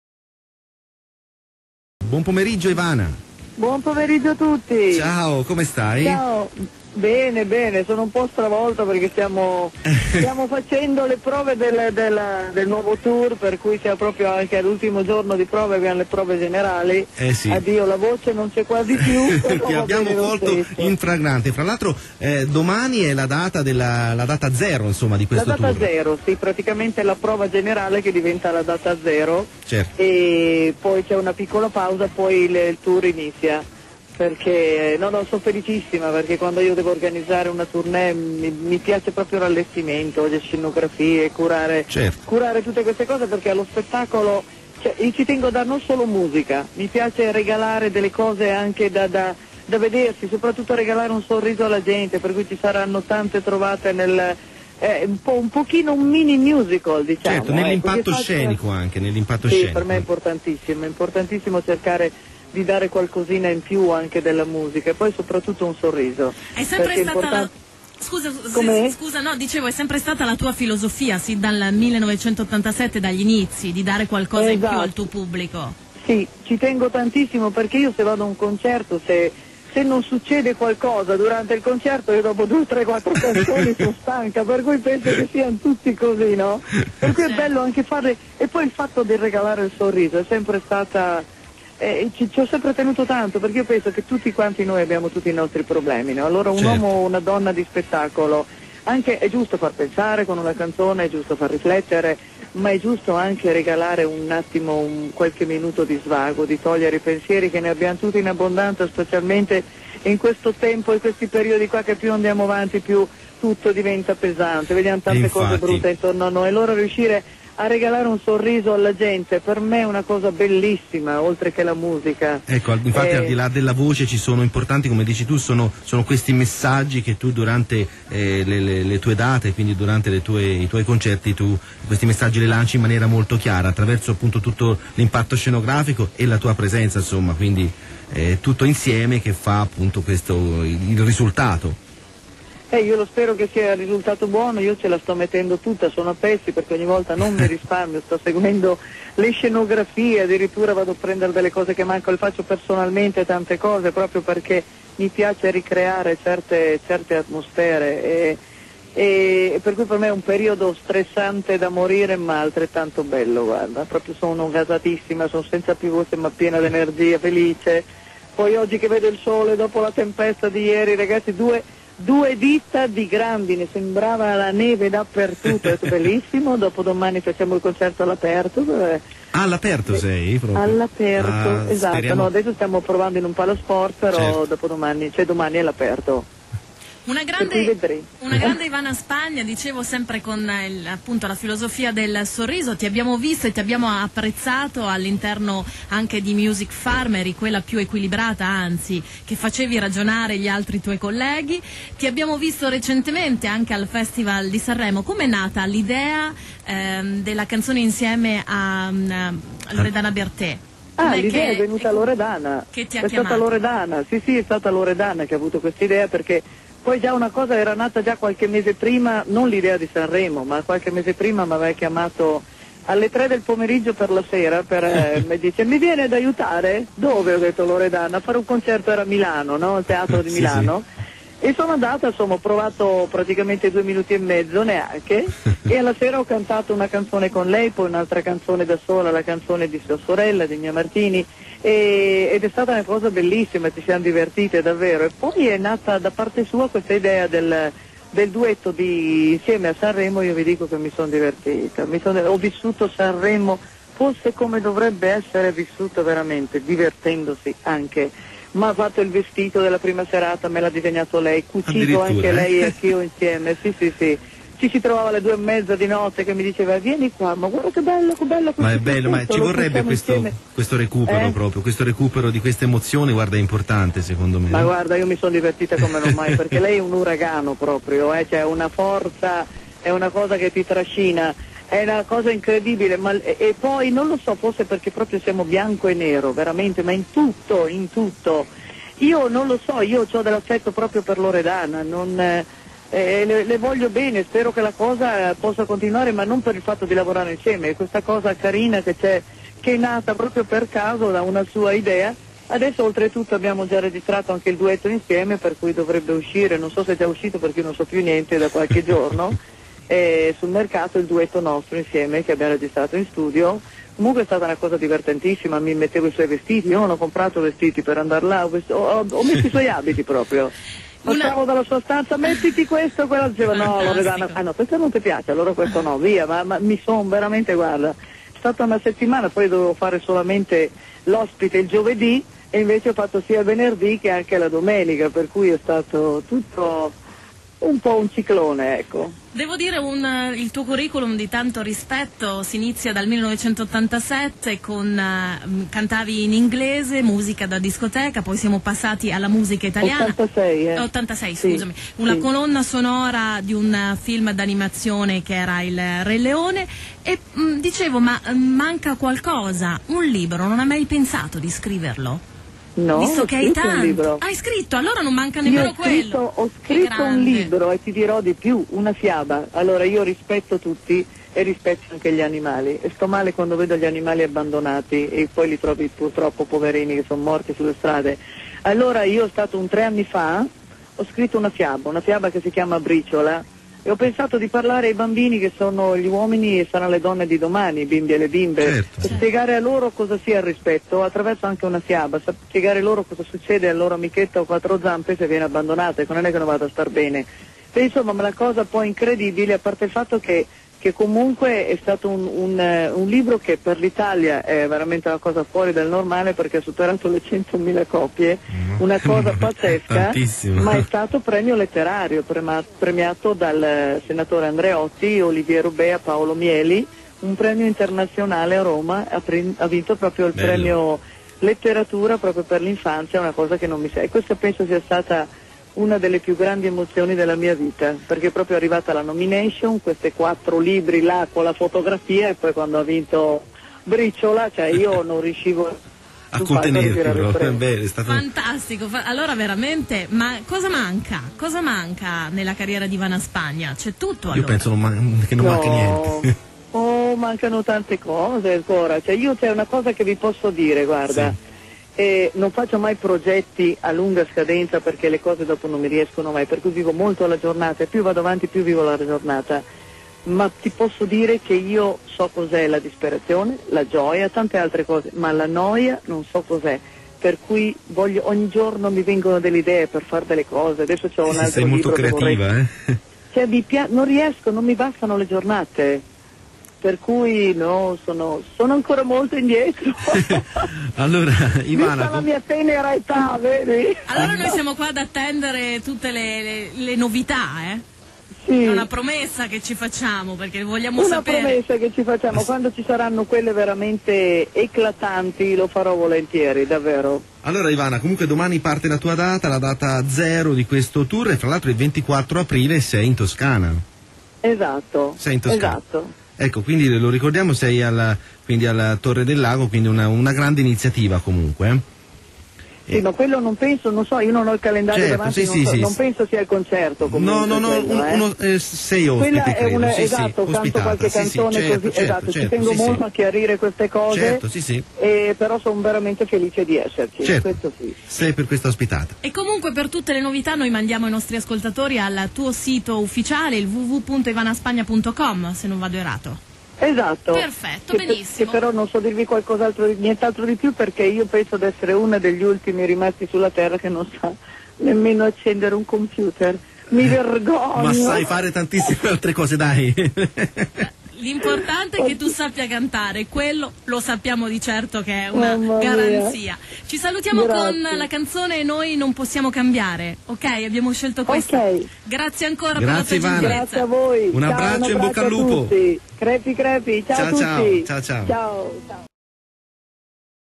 INTERVISTA TELEFONICA A IVANA SPAGNA by Enrico Ciampalini
Enrico Ciampalini e Eva Edili dagli studi di Radio Rosa intervistano Spagna. Anticipazioni del nuovo tour e dell'ospitata del 24 Aprile al Mamamia.